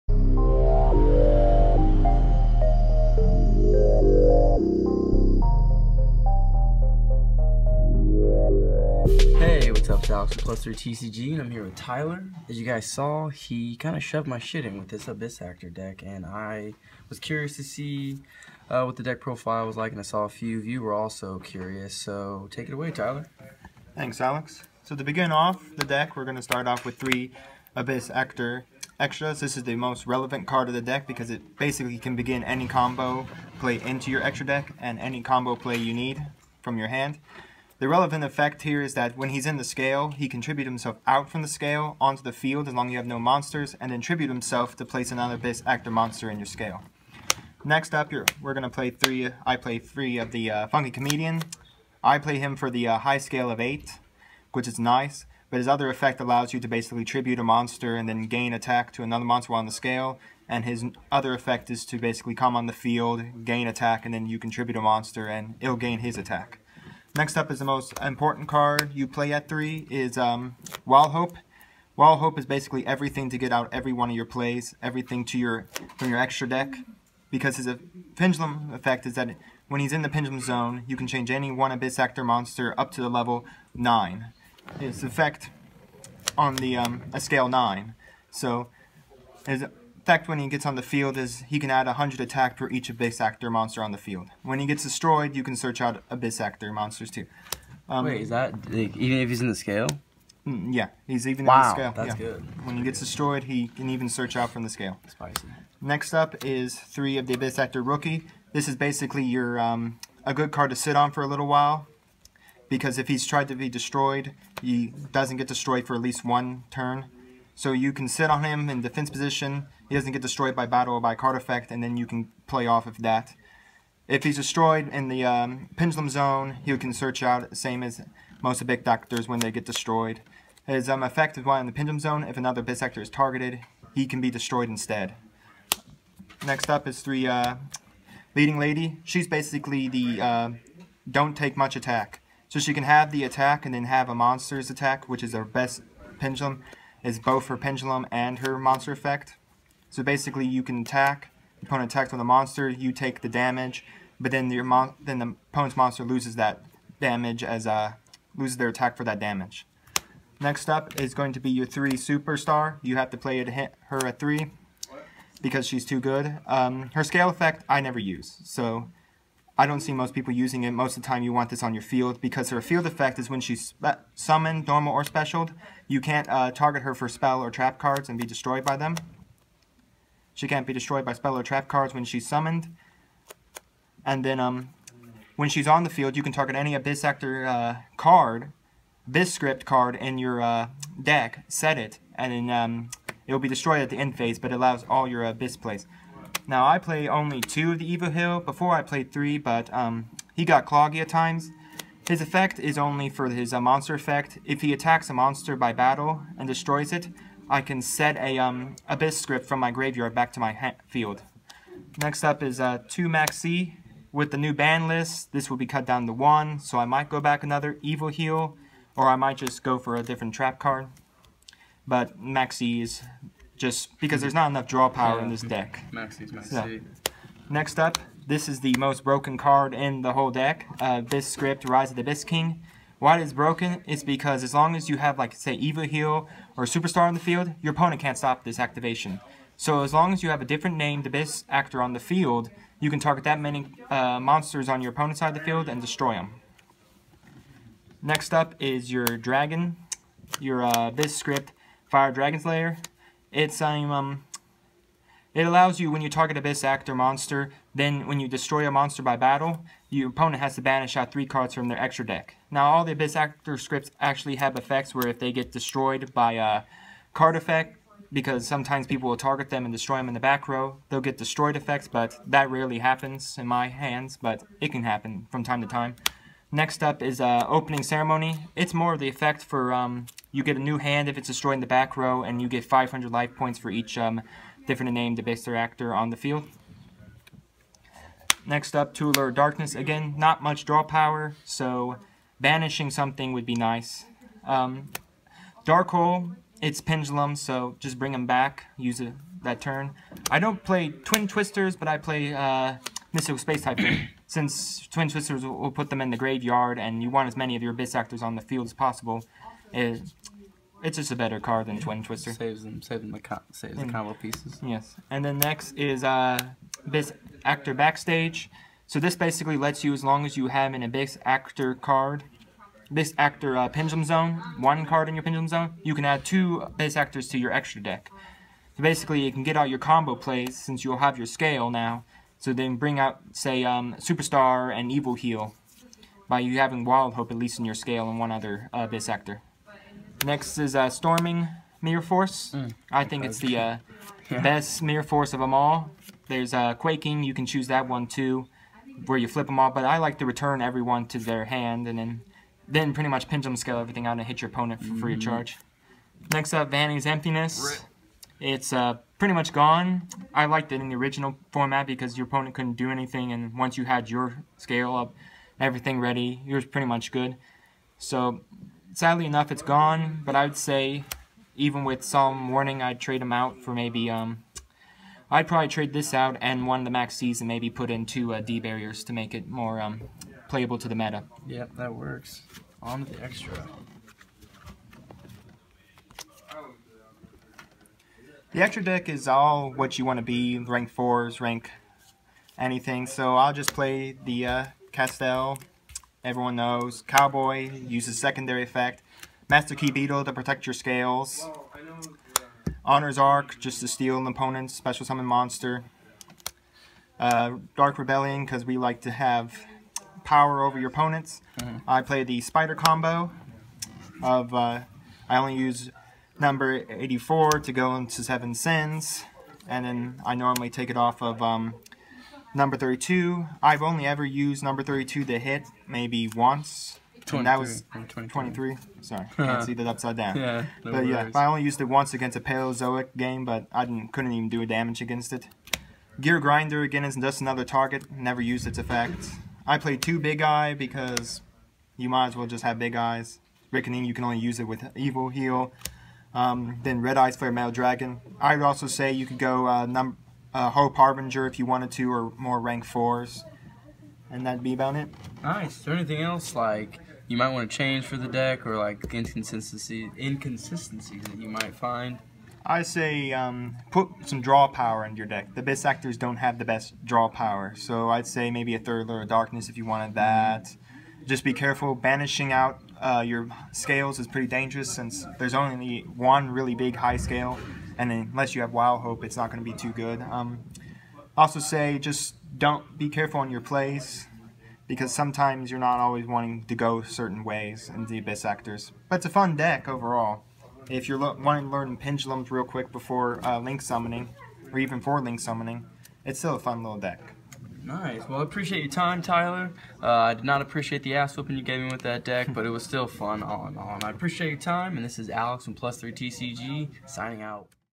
Yugioh! *BEST* Abyss Actor Deck Profile April 2017
Get ready for Team Ragnarock VS Aster Phoenix this Friday, November 2, 2018 at 4pm GMT4
Per request, Tyler and his Abyss Actor Deck Profile!